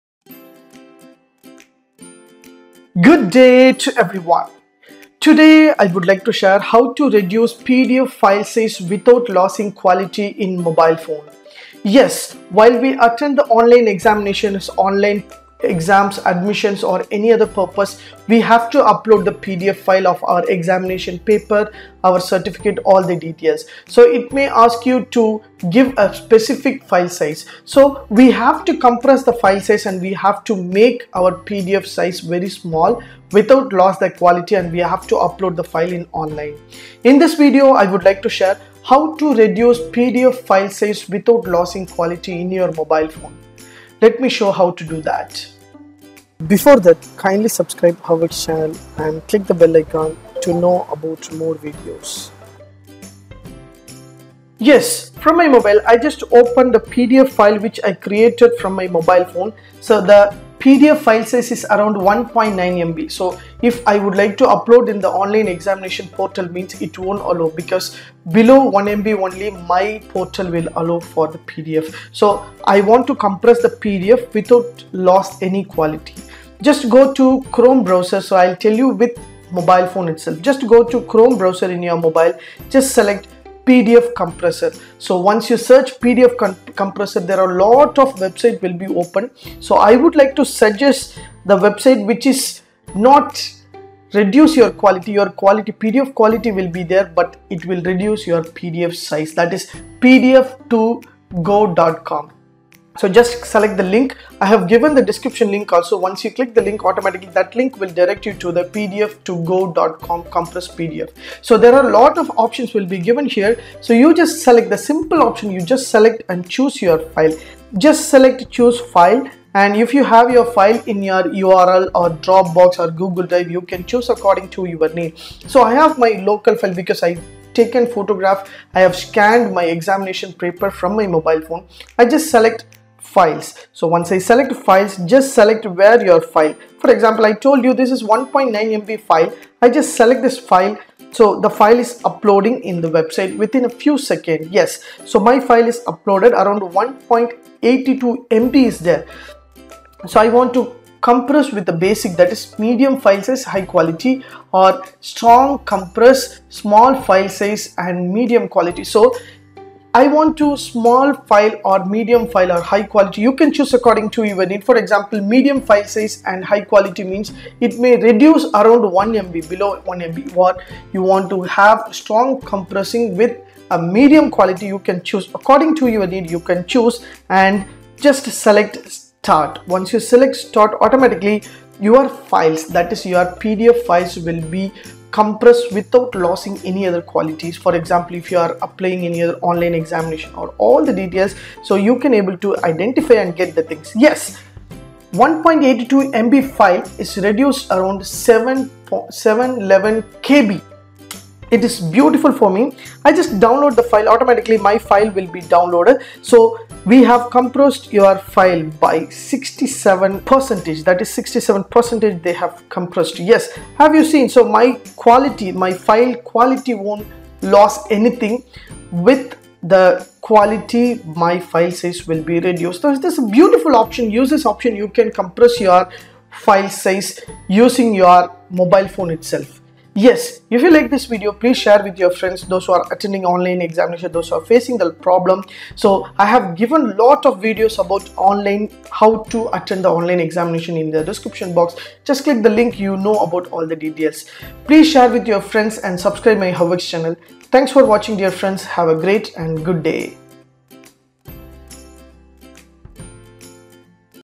How to reduce pdf file size without losing quality in mobile
This video tutorial will help you How to reduce pdf file size without losing quality in mobile. Now all exam and interview are commence from online we need to upload our certificate, exam paper,  document through online using mobile it self we can compress pdf files without losing the file quality

00:00 Introduction
01:00 How to reduce pdf file size without losing quality
03:35 Quality of the PDF after compress in mobile